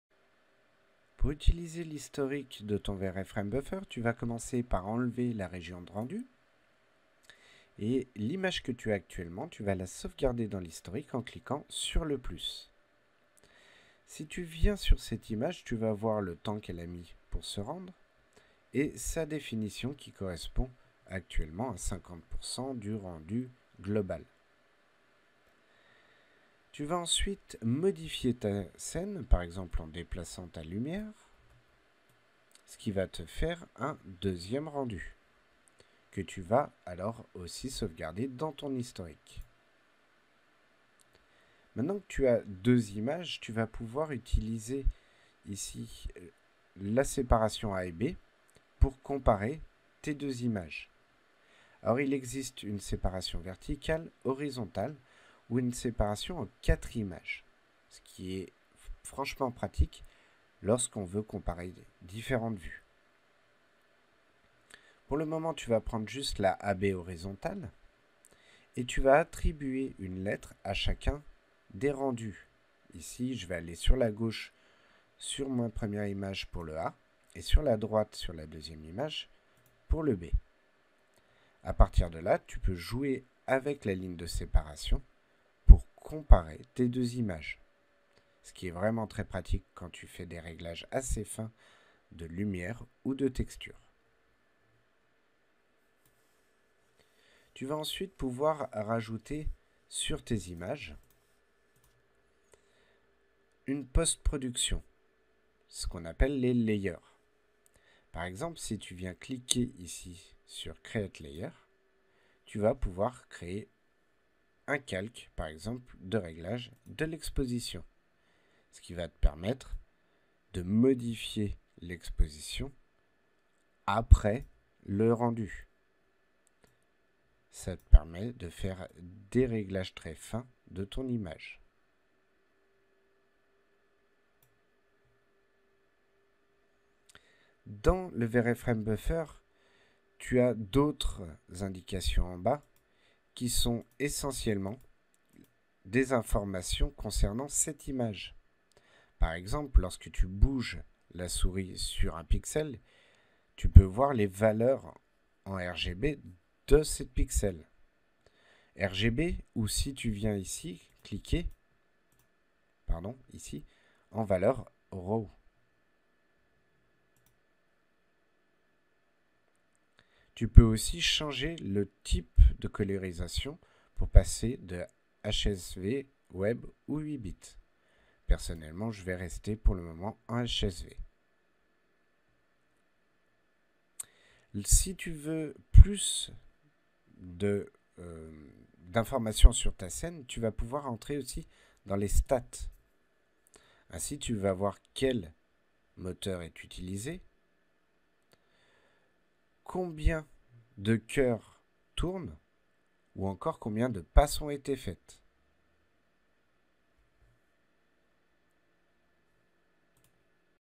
Le Frame Buffer de V-ray
Découvrez comment utiliser ce logiciel dans notre nouveau tuto en ligne.

Vous voulez en apprendre plus sur 3DS Max?
Inscrivez-vous gratuitement sur notre plateforme e-learning !